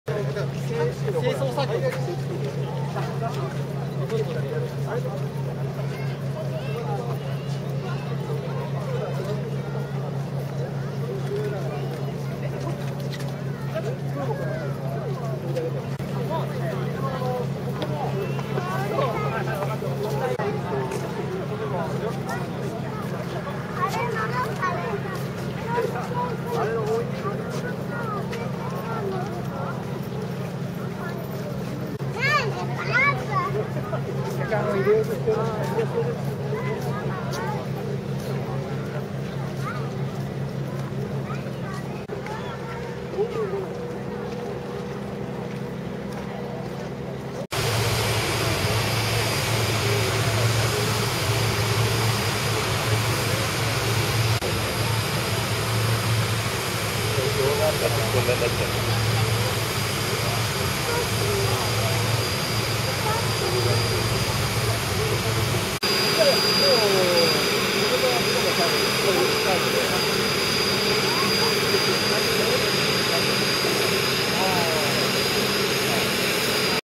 【レーダーが回転!】航空自衛隊 早期警戒管制機 Boeing E-767 AWACS aircraft,JASDF
航空自衛隊・早期警戒管制機（ＡＷＡＣＳ）Ｅー７６７（５０４号機）の地上展示。とても大きい機体なので全体を撮影するのが困難でした。サービスで丸い円盤みたいなレーダーを回転していました。横田基地 2011/8/20  Japan Air Self-Defense Force(JASDF).Boeing E-767 AWACS(airborne warning and control system)aircraft.at Yokota Air Base.Fussa City,Tokyo Metropolis.